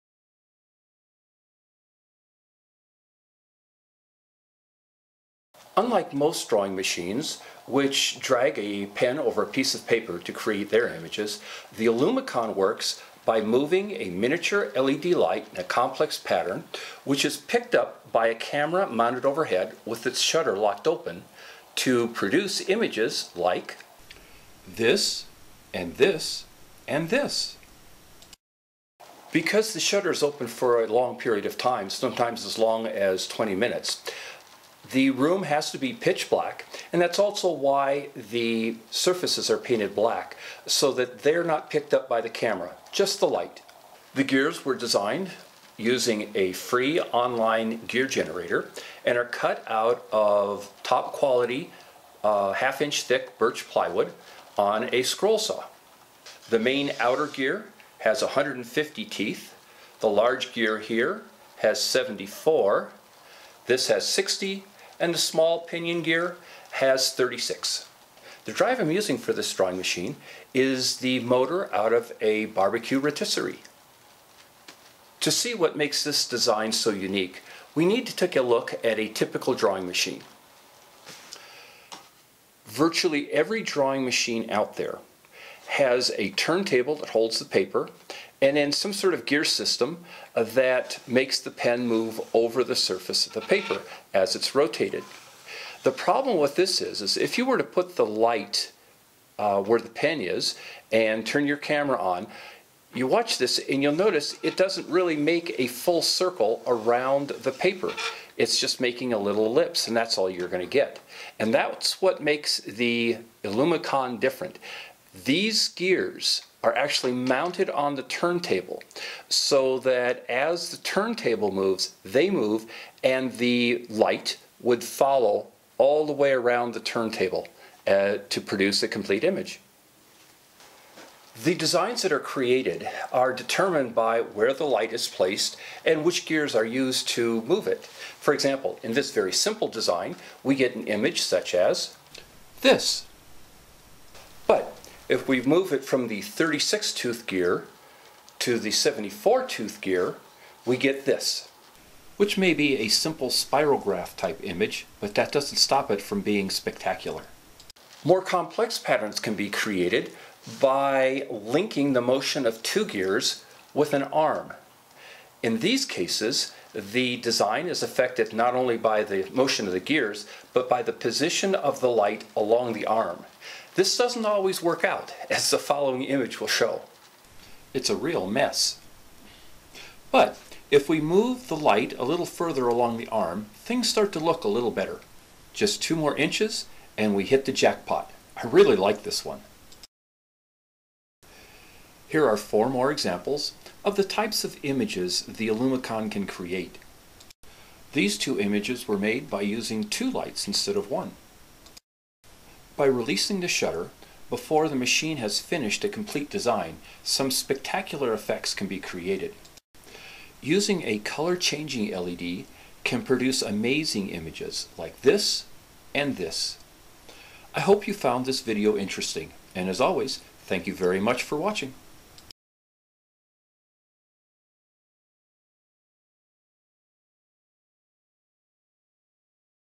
Illumicon Drawing Machine
A mechanical drawing machine that paints with light.